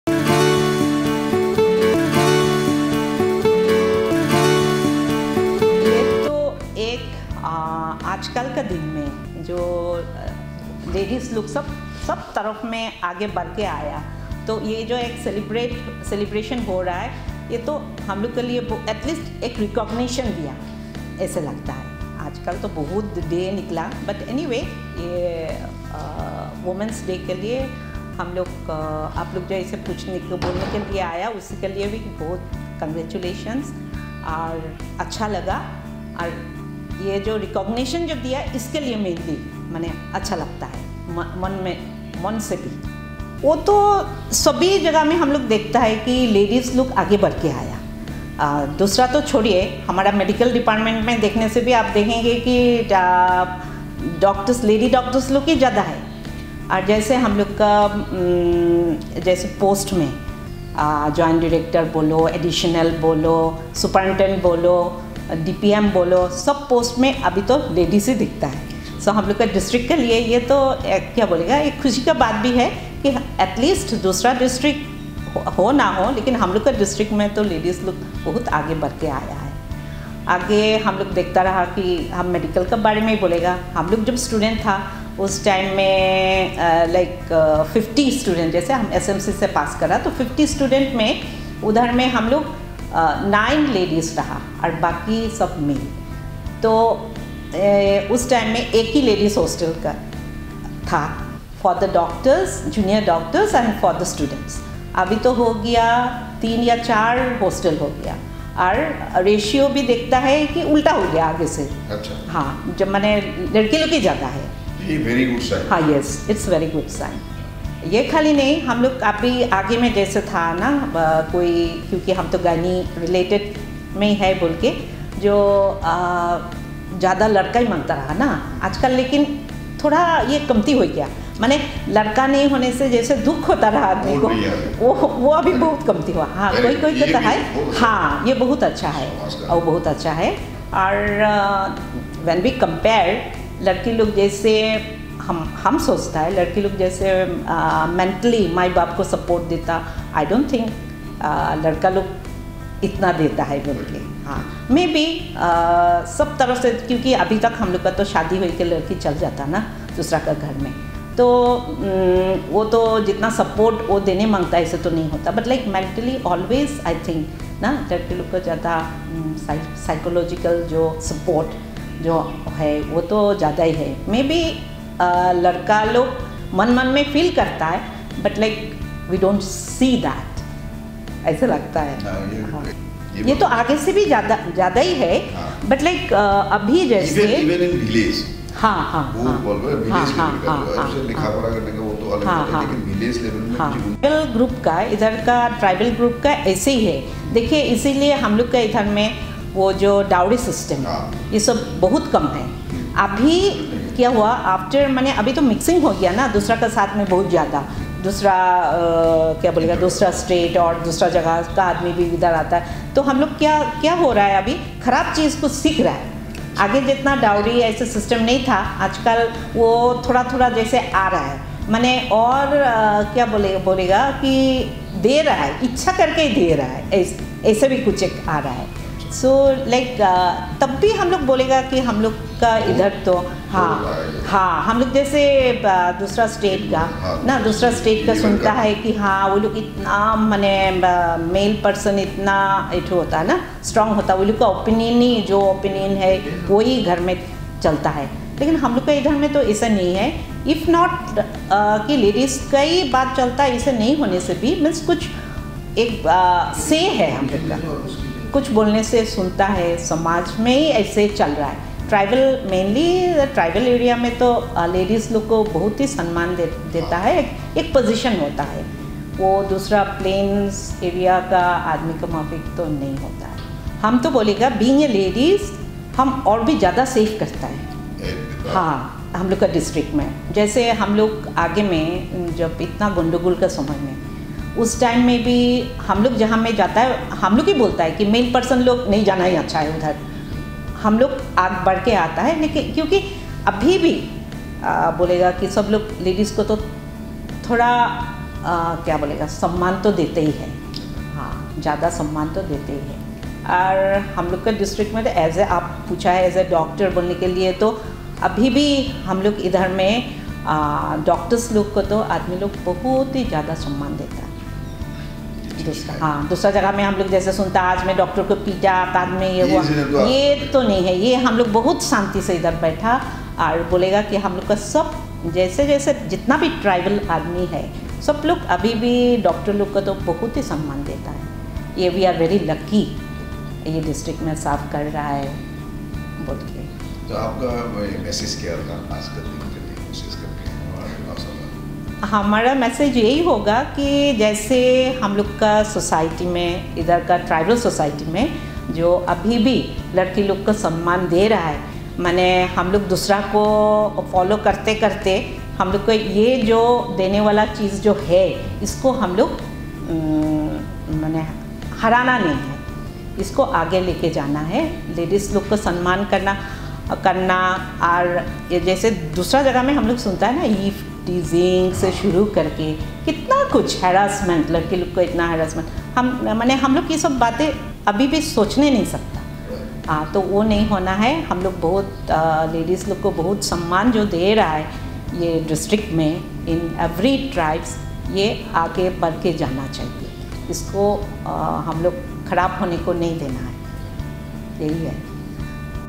International Women's Day
Joint Director of Health Services, Haflong,  Share his views with Hills Live